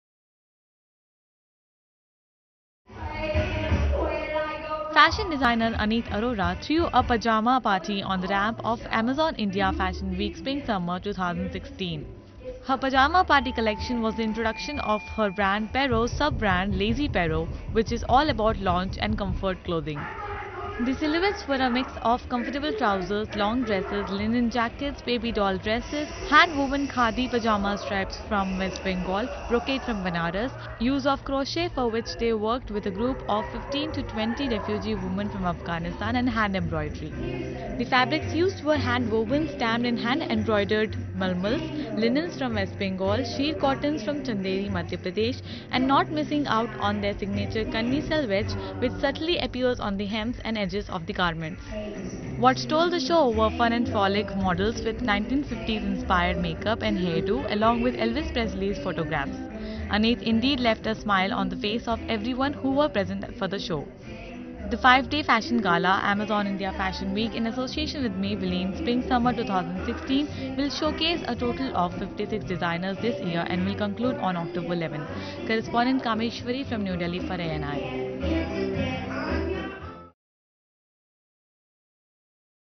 Aneeth Arora throws 'Pajama Party' on Day 4 of Amazon India Fashion Week SS'16
New Delhi, Oct 11 (ANI): Fashion designer Aneeth Arora threw a pajama party on the ramp of Amazon India Fashion Week Spring Summer 2016. Her 'Pajama Party' collection was the introduction of her brand 'Pero's sub-brand, 'Lazy Pero', which is all about lounge and comfort clothing. The silhouettes were a mix of comfortable trousers, long dresses, linen jackets, baby doll dresses, hand-woven khadi pajama stripes from West Bengal, brocade from Banaras, use of crochet for which they worked with a group of 15 to 20 refugee women from Afghanistan and hand embroidery. The fabrics used were hand-woven, stamped and hand-embroidered mulmuls, linens from West Bengal, sheer cottons from Chanderi Madhya Pradesh and not missing out on their signature kanni selvedge, which subtly appears on the hems and edges of the garments. What stole the show were fun and frolic models with 1950's inspired makeup and hairdo along with Elvis Presley's photographs. Aneeth indeed left a smile on the faces of everyone who were present for her show. The five-day fashion gala, Amazon India Fashion Week in association with Maybelline Spring Summer 2016 will showcase a total of 56 designers this year and will conclude on October 11.